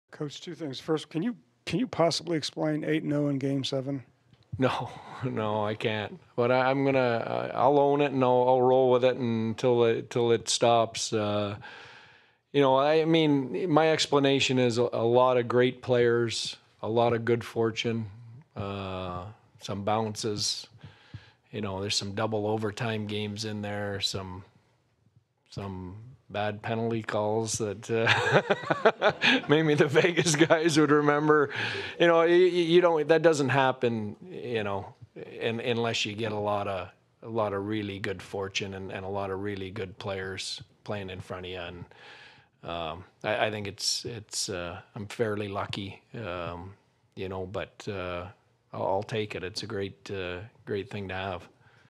Pete DeBoer on having an 8:0 coaching record in Game 7s / 5.05.2024
"I'm going to own it, and I'm going to ride with it until it stops."